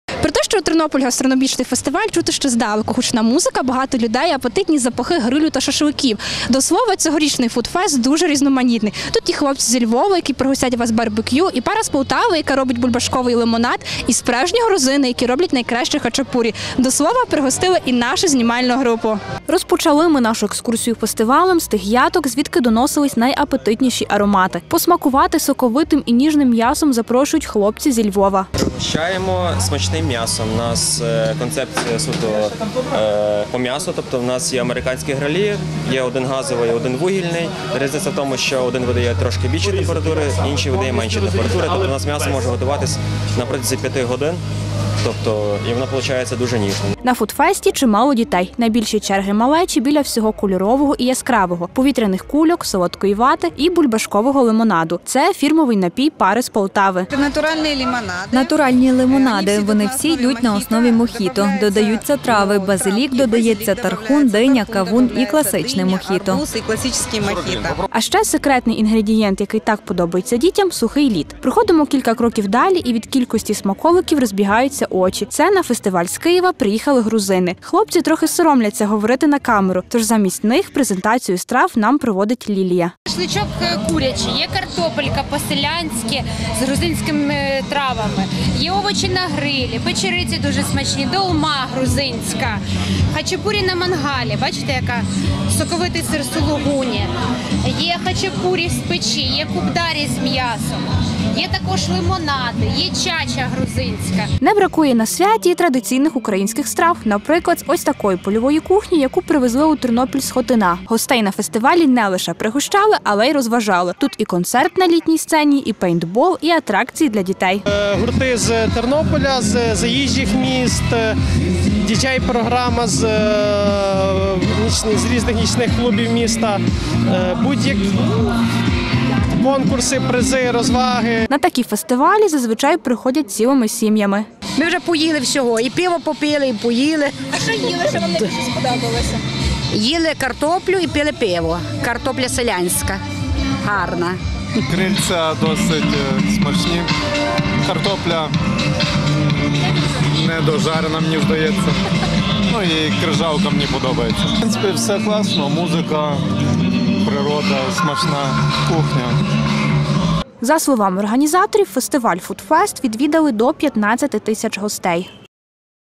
У Тернополі відбувся «Ternopil Food Fest»
Музика, розваги і дуже багато їжі. Це про гастрономічний фестиваль «Ternopil Food fest». Окрім, місцевих рестораторів, кулінарними вміннями дивували і кухарі з різних куточків України.  Смачний фестиваль відвідала і наша Діана Гвоздь.